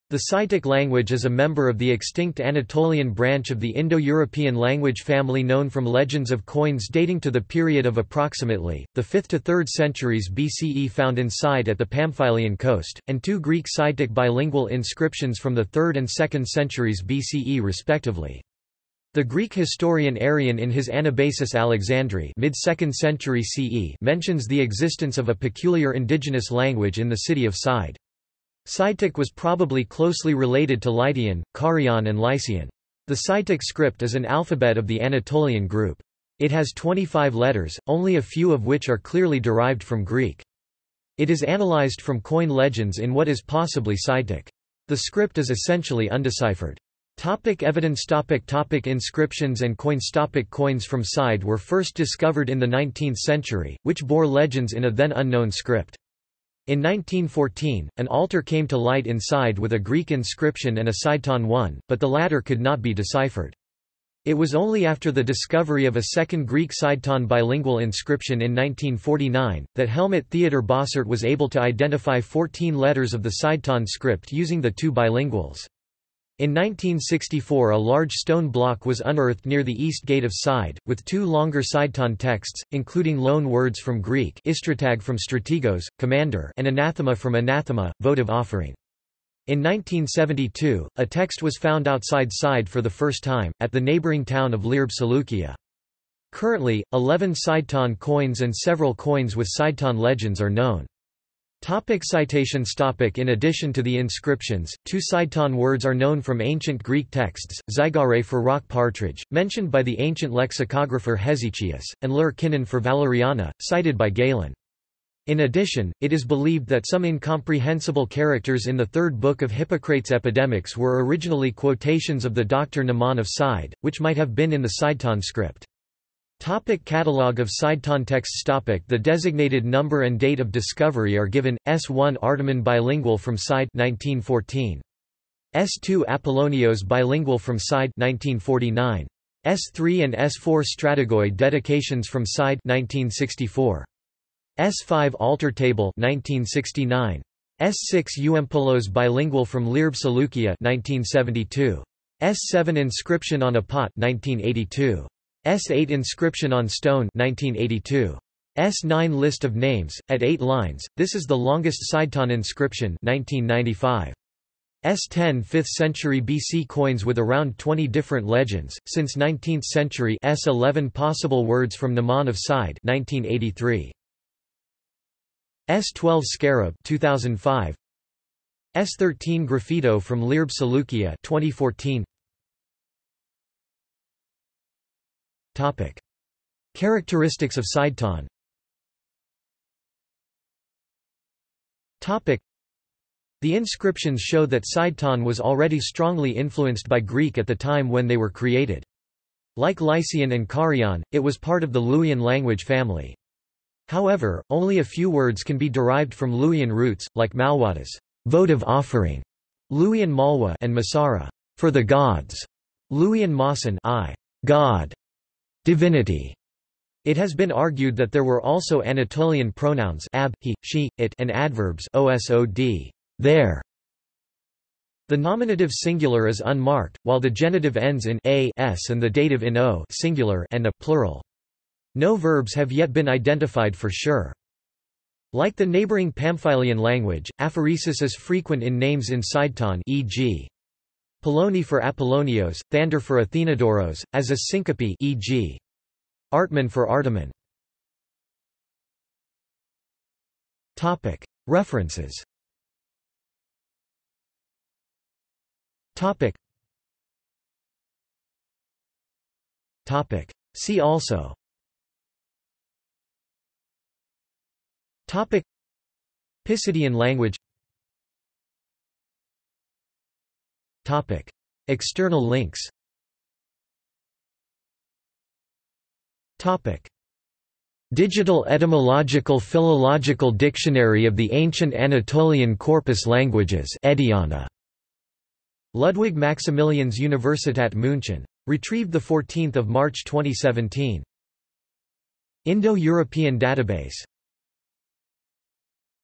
Sidetic script | Wikipedia audio article
This is an audio version of the Wikipedia Article:
Sidetic script

SUMMARY
The Sidetic language is a member of the extinct Anatolian branch of the Indo-European language family known from legends of coins dating to the period of approx. the 5th to 3rd centuries BCE found in Side at the Pamphylian coast, and two Greek–Sidetic bilingual inscriptions from the 3rd and 2nd centuries BCE respectively. The Greek historian Arrian in his Anabasis Alexandri (mid-2nd century CE) mentions the existence of a peculiar indigenous language in the city of Side.
Sidetic was probably closely related to Lydian, Carian and Lycian.
The Sidetic script is an alphabet of the Anatolian group. It has   25 letters, only a few of which are clearly derived from Greek. It is analysed from coin legends in what is possibly Sidetic. The script is essentially undeciphered.